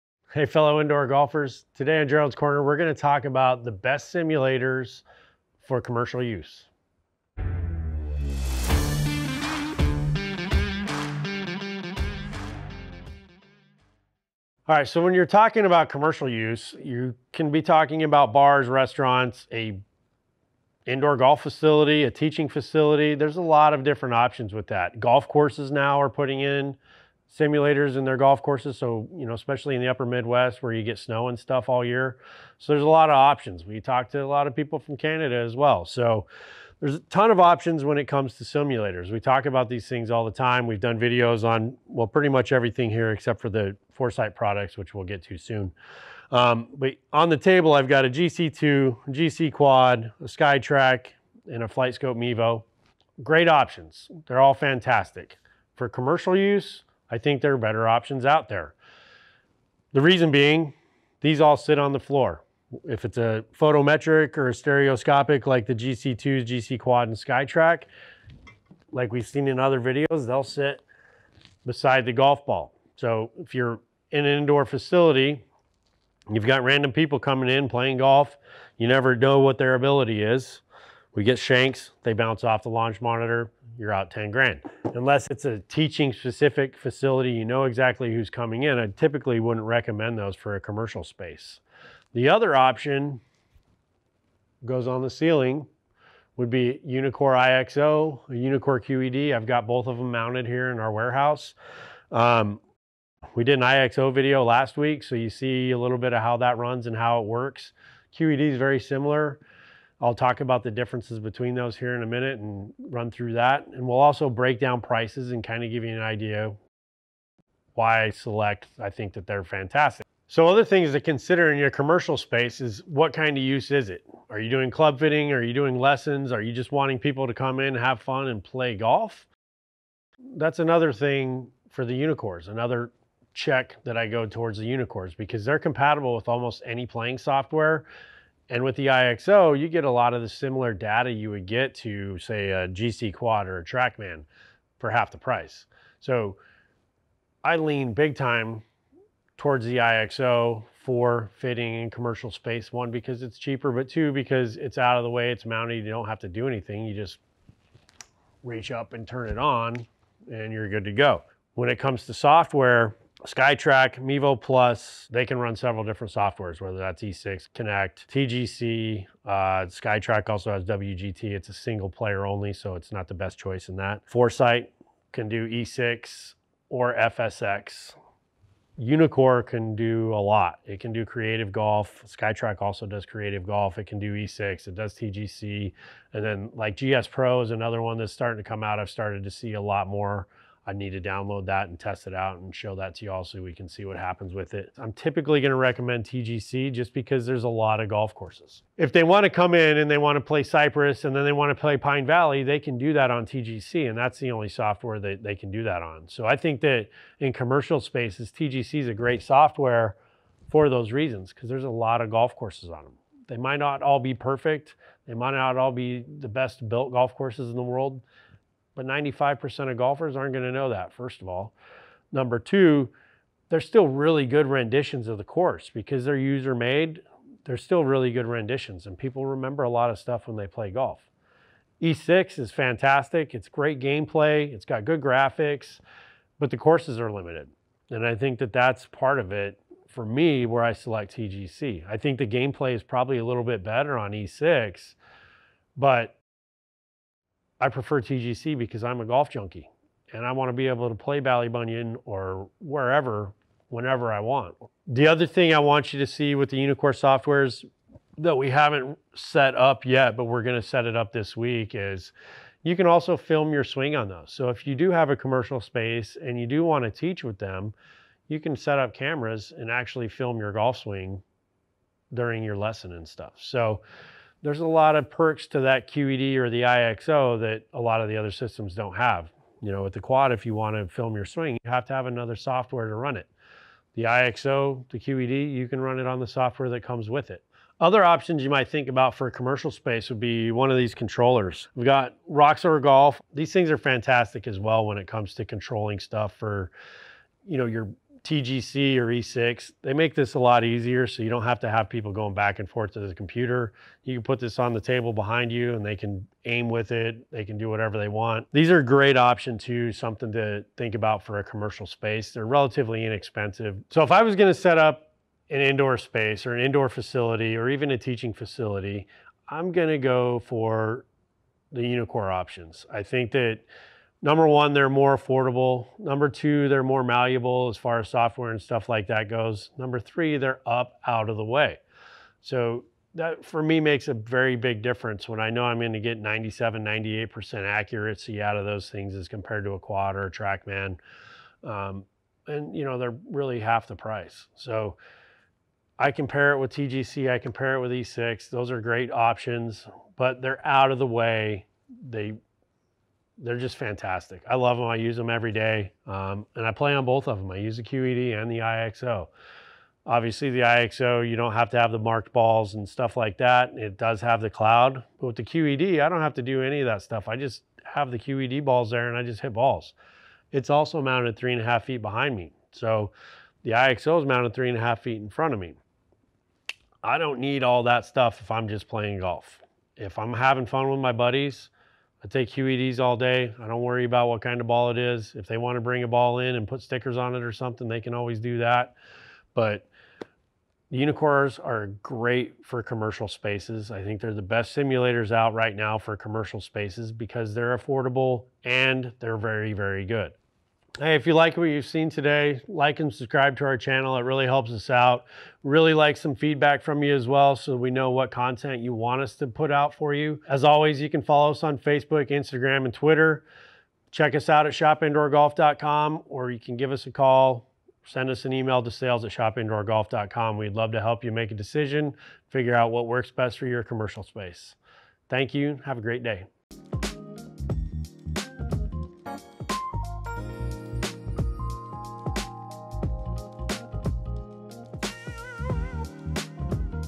Best Launch Monitor For Commercial Use
Hey fellow indoor golfers. In this episode of Gerald's Corner we talk about the best launch monitors for use in an indoor commercial facility. We cover the different types of launch monitors, as well as, our preferred software packages. We also cover the differences of the Skytrak , Flightscope, Uneekor , and Foresight products and their own respective capabilities.

00:15 -Different launch monitor options
02:26 -What type of commercial space do you have
03:08 -Software compatibilities
03:45 -SIG preferred softwares
04:42 -Other launch monitor options
05:42 -SIG recommendations & pricing

and hit up our chats or give us a call at and we can answer any questions you might have from there.